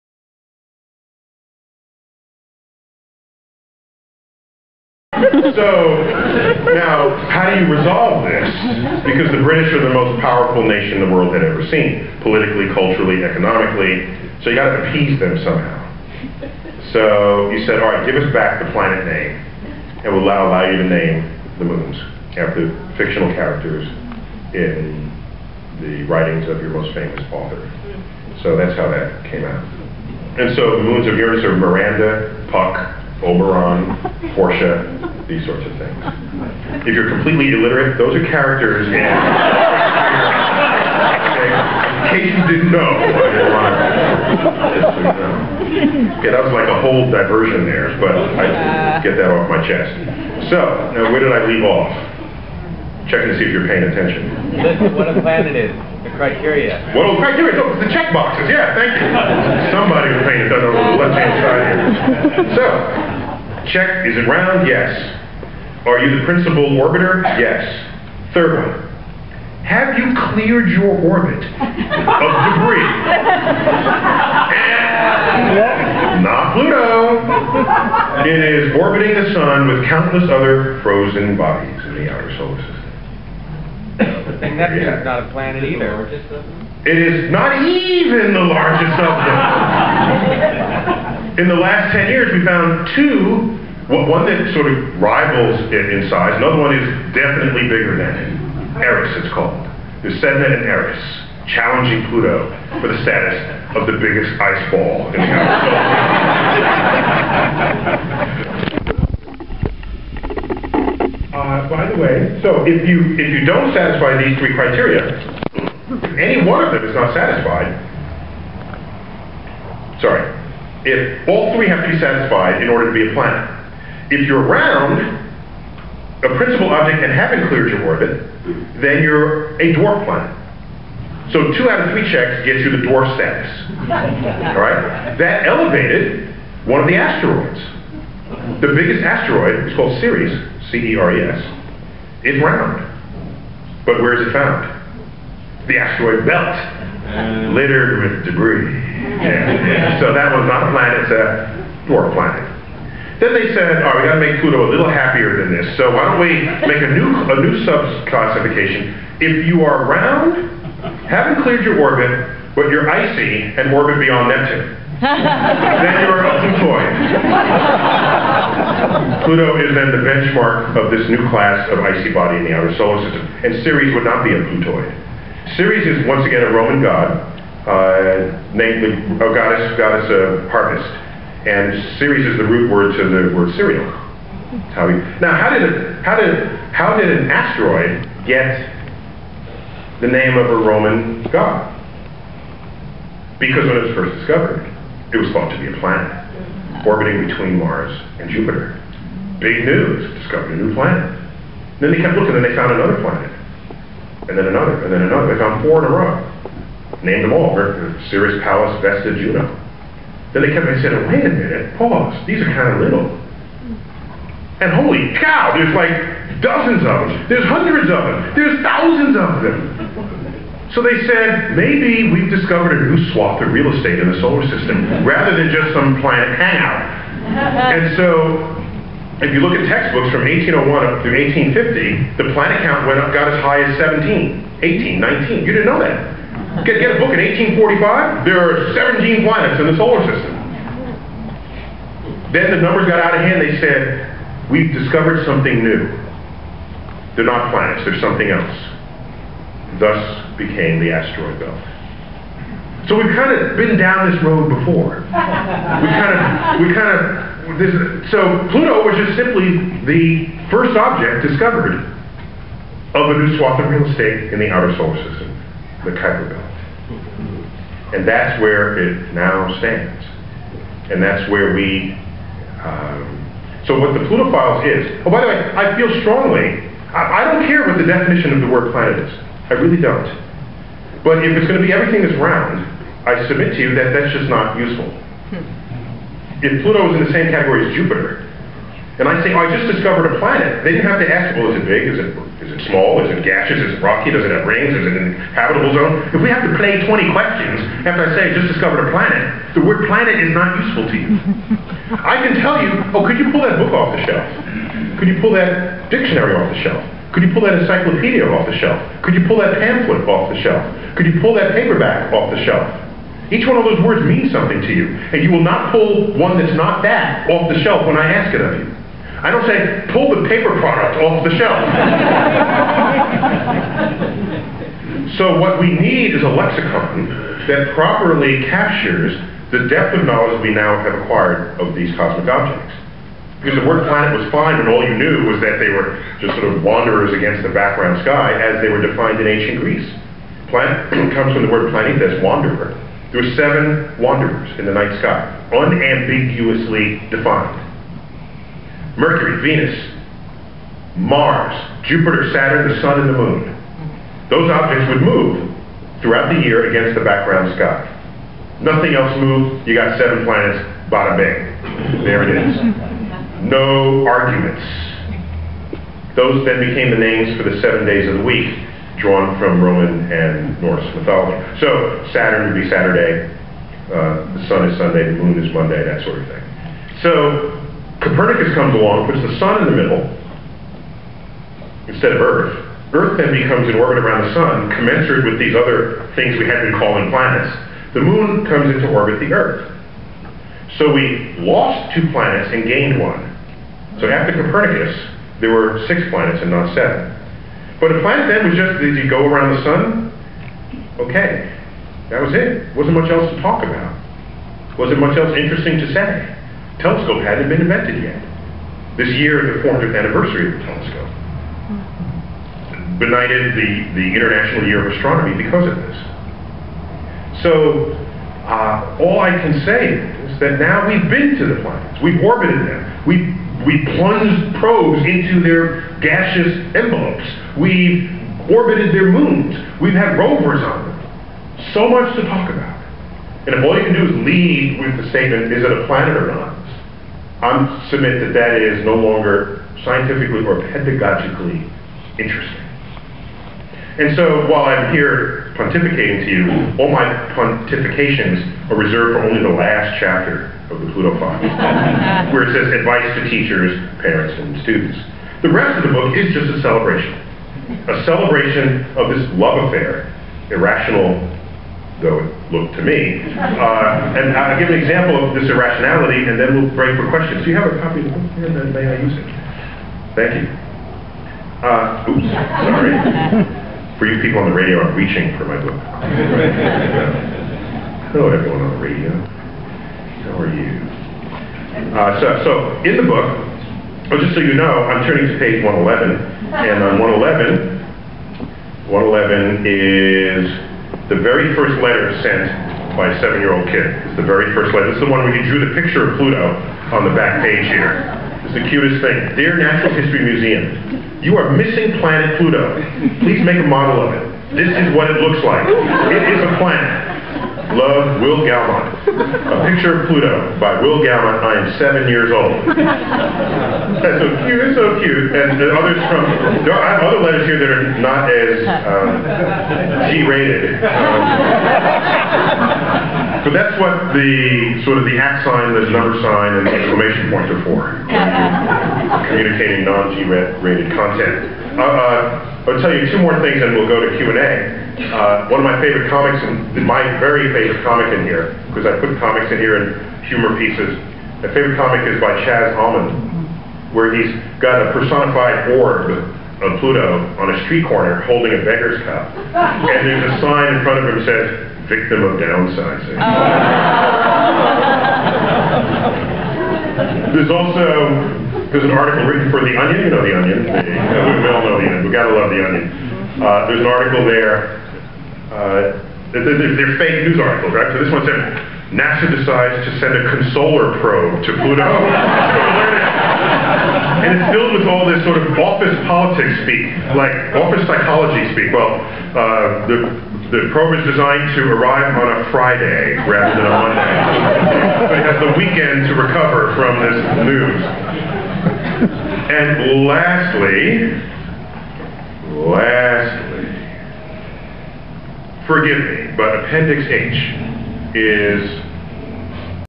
Neil deGrasse Tyson discusses The Pluto Files @ The Commonwealth Club - 02/05/09 - Part 3 of 7
In support of his book The Pluto Files, Neil DeGrasse Tyson discusses the chain of events that led to Pluto's fall from grace at the Commonwealth Club in San Francisco, CA.  Part 3 of 7.